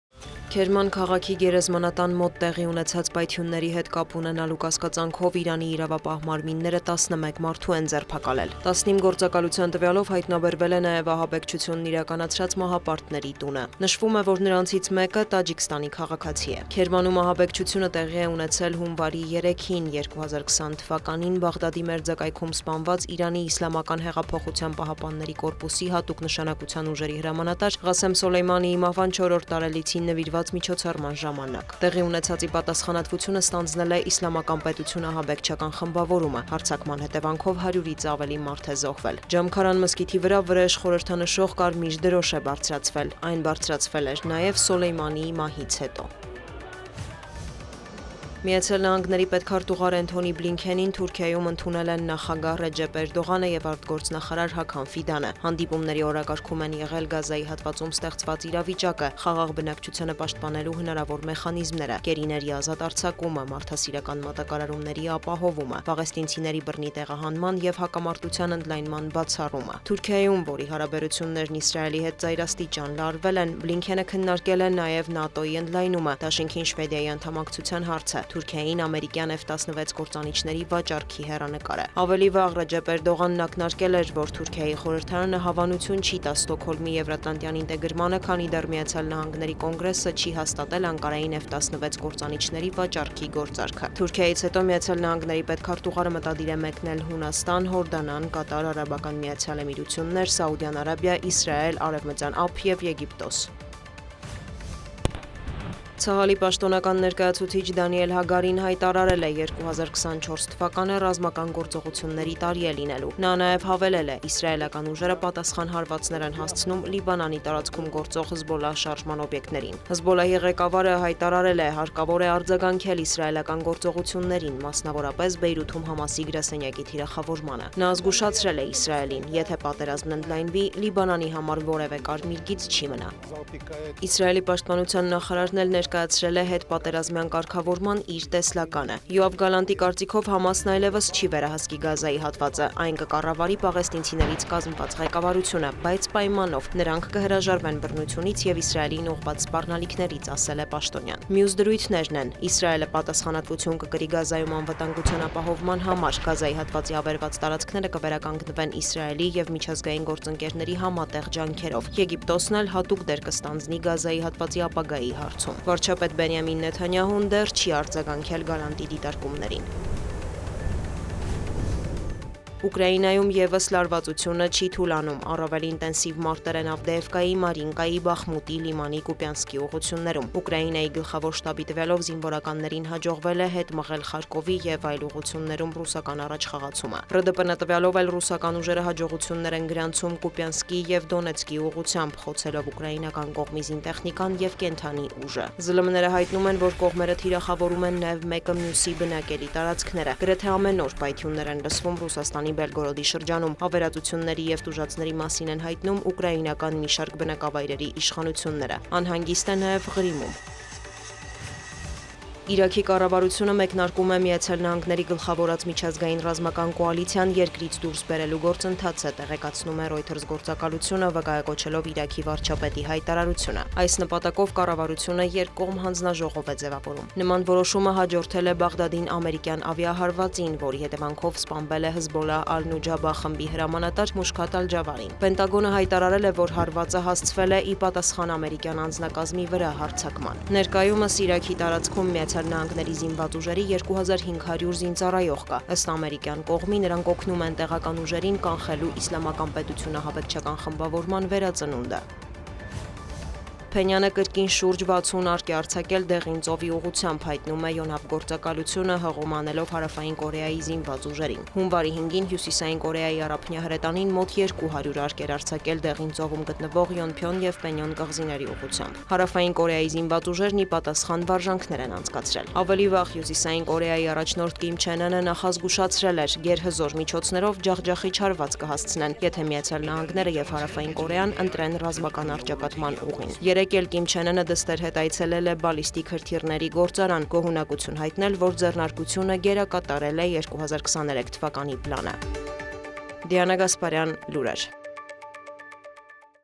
Եթե պատերազմն ընդլայնվի, Լիբանանի համար կարմիր գիծ չի մնա. Հագարի
🔻 Ռուս-ուկրաինական լարվածությունը չի թուլանում
🔻 Մեկնարկում է ԱՄՆ գլխավորած միջազգային ռազմական կոալիցիան Իրաքից դուրս բերելու գործընթացը